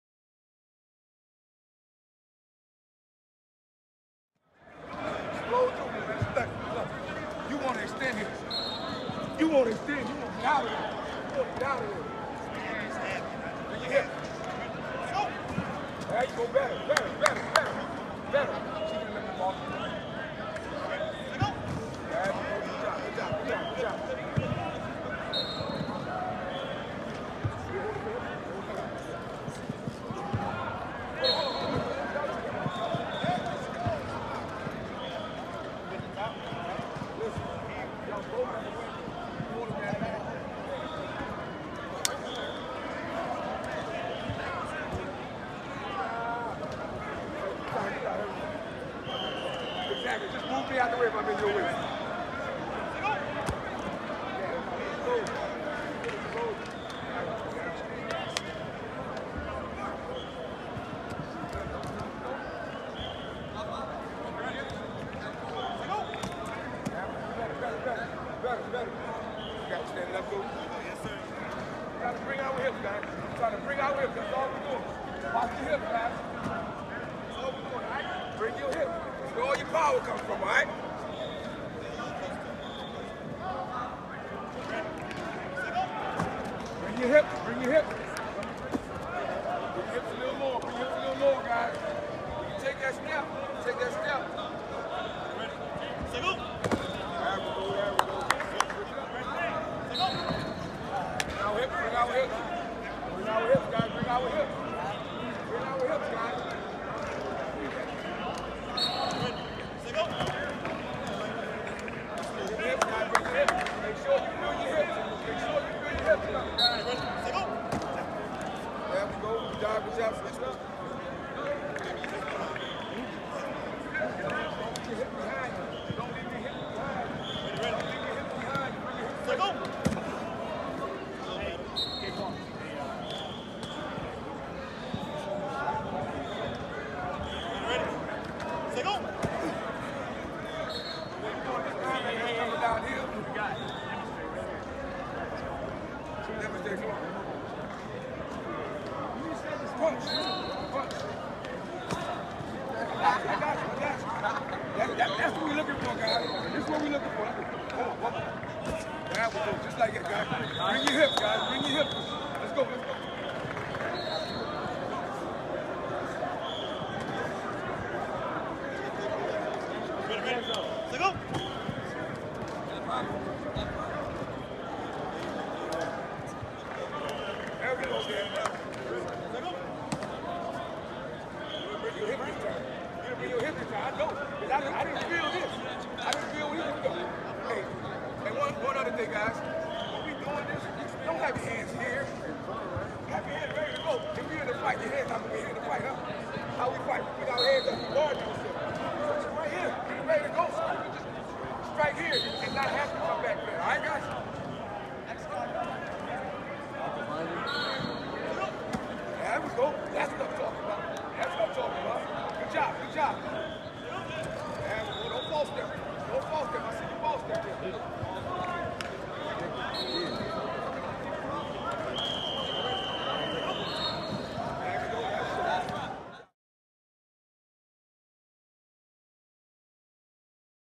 Kevin Faulk works with RB prospects at LSU Skills Camp (June 6, 2021)
Rising sophomore running back Josh Williams assists Tigers' position coach to demonstrate drill.